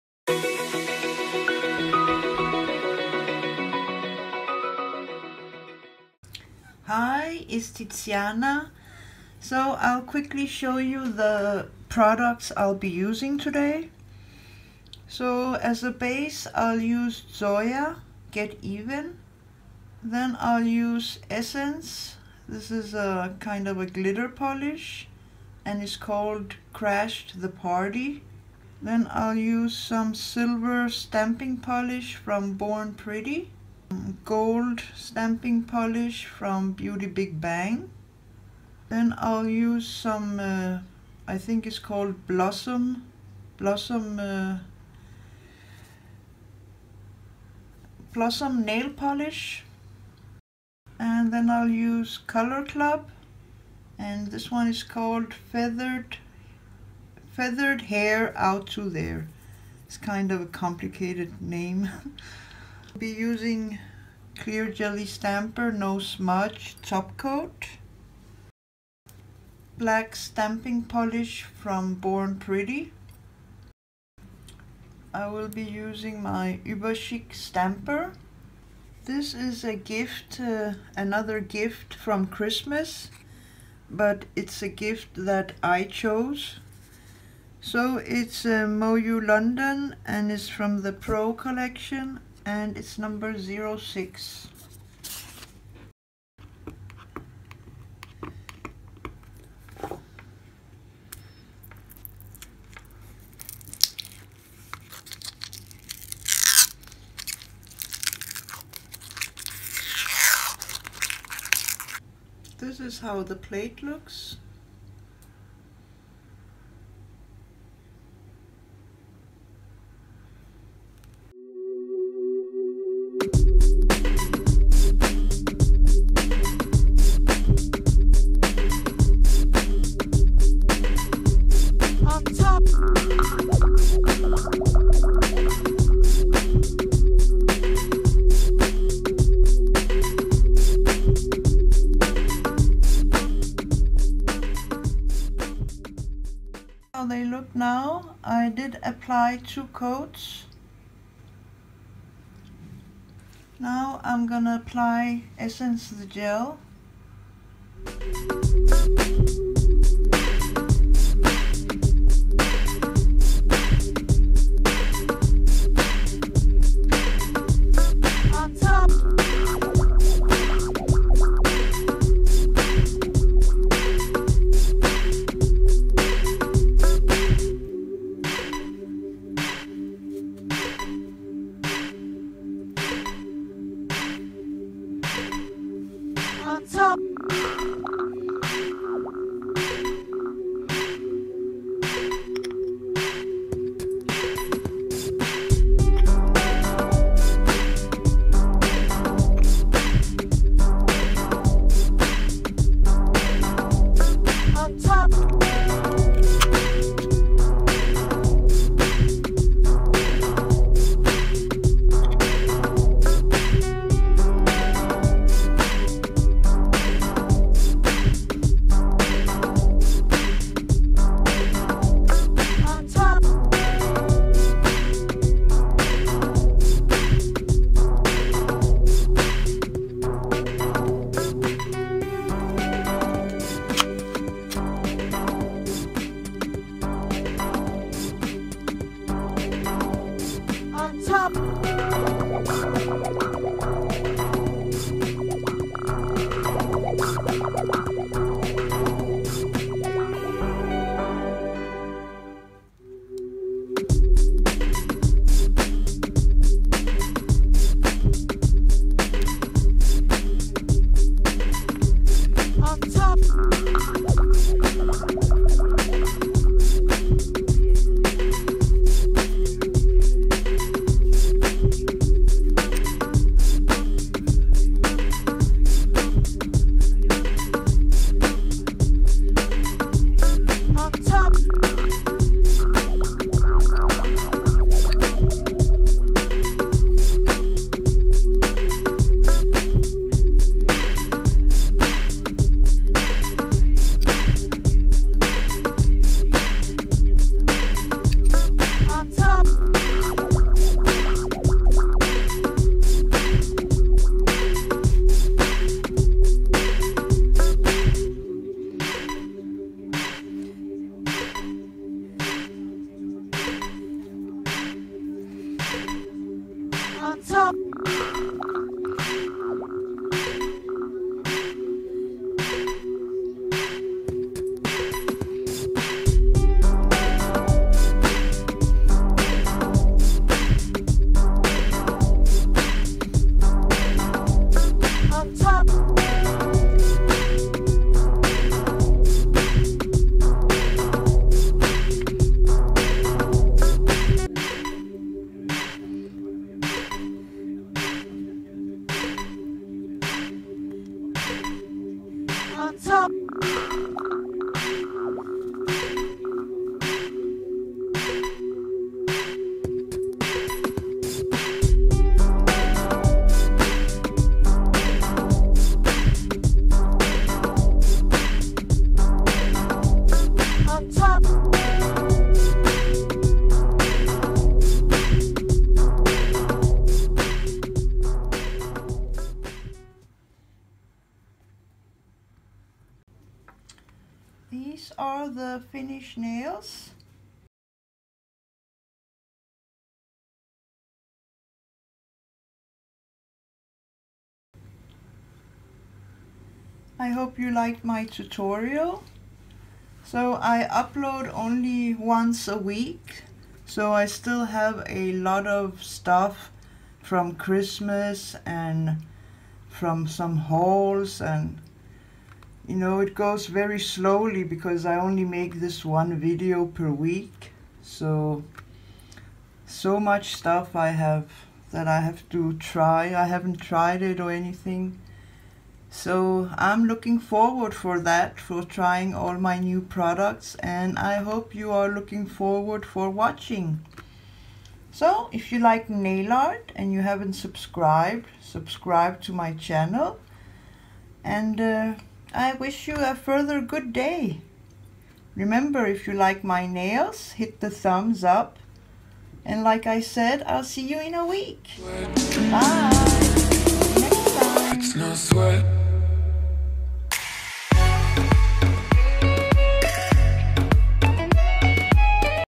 Moyou London Pro - Essence - Color Club
Hi , today i will be doing a pink flower design whit a new Moyou London plate i got for christmas, i will use Essence glitter topper-crash the party- and.

Products used:
Moyou london pro. 06 stamping plate
Color Club - Feathered hair out to there
Born Pretty silver stamping polish
BeautyBigBang gold stamping polish
Born Pretty black stamping polish
CJS no smudge top coat
Essence - Crash the party
Uber chic stamper

For reviews of products: Leave a comment  on my about section on my channel and i will give you
my email and address